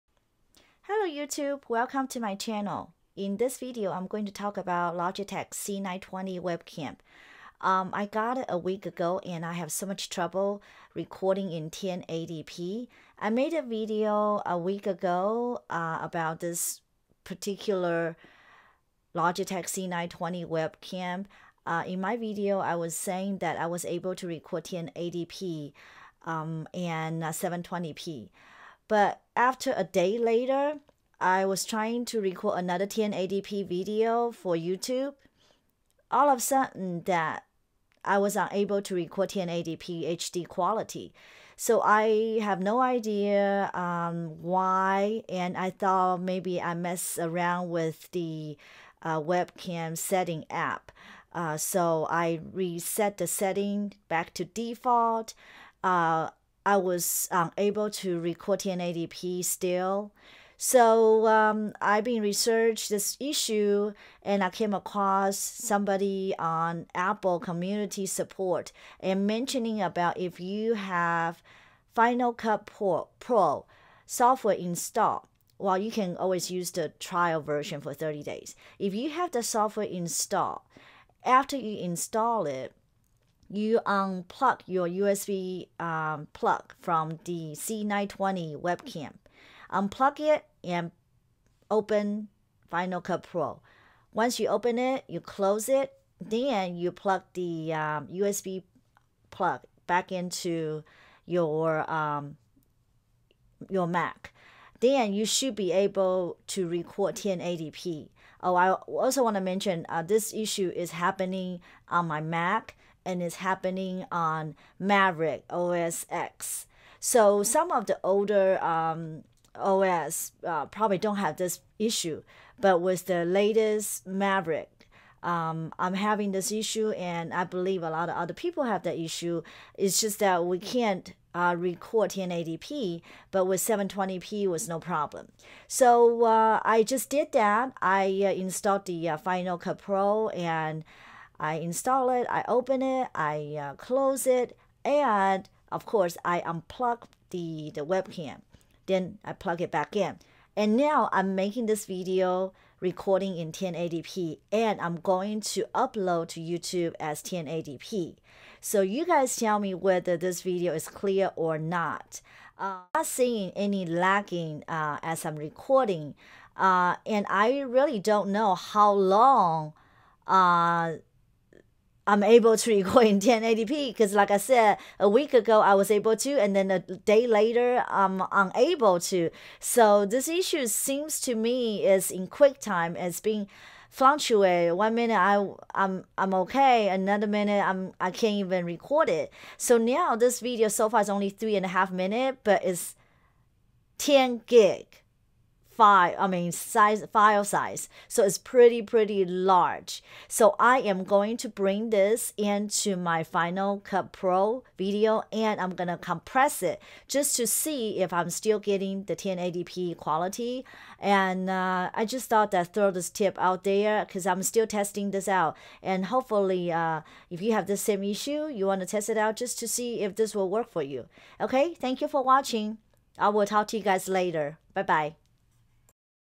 Can Logitech C920 record 1080p on Mac OSX Mavericks?
UPDATE: In order to record 1080p quality, first install QT 7 Pro and then you can record 1080p high quality video in QT 10 and you won't see any delay or lagging.

I also found some inconsistent results, if QT is lagging, I would open the Final Cut Pro first to activate the codec then unplug webcam USB and then plug it back in to your Mac before start recording in QT.

Logitech C920 1080p recording issue on Mac OSX Mavericks
I found this solution on Apple Community Support and this was provided by Graham Lucas
1. Install the Final Cut Pro X Trial
2, Open FCP, click OK to use it as a 30 day trial, then quit FCP.
3. Unplug and replug the C920 from the USB port.
4. Open Quicktime Player, New Movie Recording, set the quality to Maximum and the resolution is now 1080p.

Why this works: FCP installs the Apple ProRes Codec so Quicktime will display the C920's video in Apple ProRes 4444, 1920x1080 format.

Note:
When you record in max quality in quick time, the file size is very large. you can compress the file first before you upload it to Youtube (using Compressor). You will not lose the video quality after compress the file.